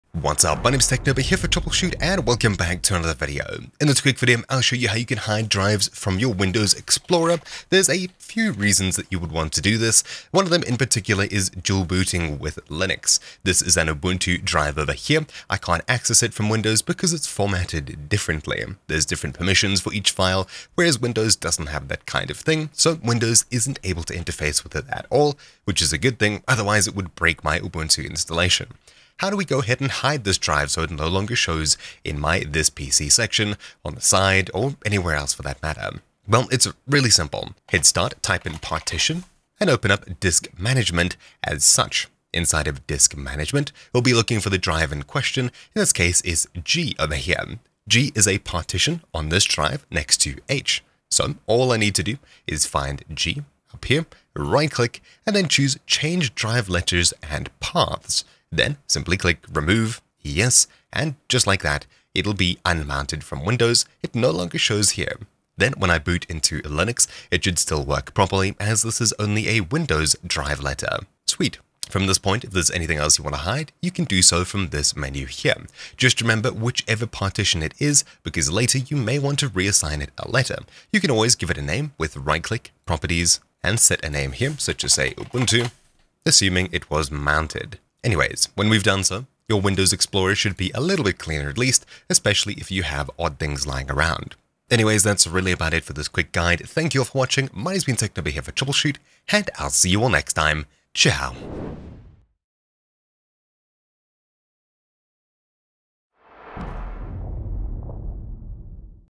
Hide Drives / Change Drive Letters | Windows Explorer Guide
Want to hide a drive in Windows Explorer or change a drive letter? This video tells you everything you need to know about disk management and Windows Explorer.

Timestamps:
0:00 - Explanation
0:35 - Hide Drives in Windows Explorer
1:15 - Unhide/Show Drives in Windows Explorer